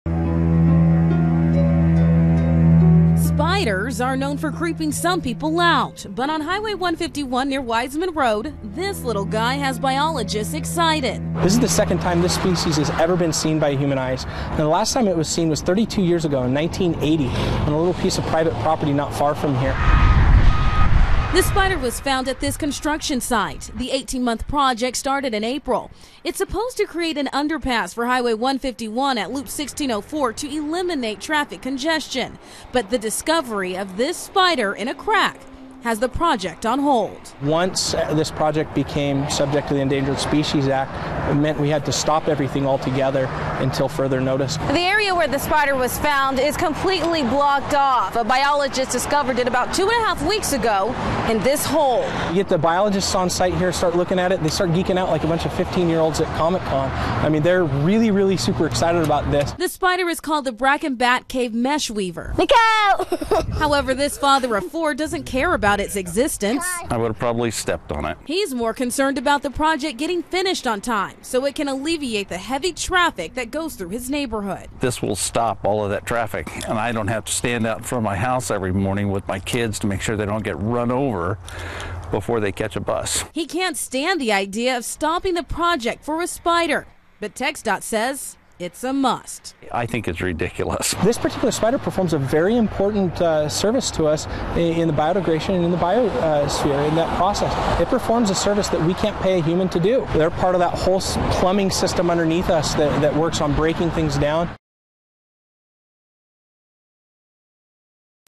Rare Spider Shuts Down Huge Construction Project
By: Christina ColemanA rare spider called the Braken Bat Cave Meshweaver was found at a construction site near Highway 151 and Wiseman Road.  The 15.1 million dollar project will be put on hold until the U.S. Fish and Wildlife Service and the Federal Highway Administration considers ways to possibly continue the project without disrupting the spiders potential habitat.The Texas Department of Transportation had biologists on site since it was determined that the area has lots of cracks that would possibly have a variety of organisms in them. Texas Department of Transportation spokesperson Josh Donat says the project became subject to the Endangered Species Act once the rare spider was discovered, "This is the second individual spider that's ever been found. It's not like we found 1 of 5,000 individuals in a species.  This is the second time this species has ever been seen by human eyes.  The last time it was seen was 32 years ago in 1980 in a little piece of property not far from here."The discovery could stop the project all together.  Several residents who live in the North San Antonio Hills neighborhood say they're frustrated with the situation.  John Mellow has lived in the neighborhood for two years now. He says his neighborhood is busy with traffic without the project complete, "This will stop all the traffic and I don't have to stand out in the front of my house every morning with my kids and make sure they don't get run over before they catch the bus.Officials are not sure if and when the project will continue.Follow Christina on Twitter: @ChristinaFox29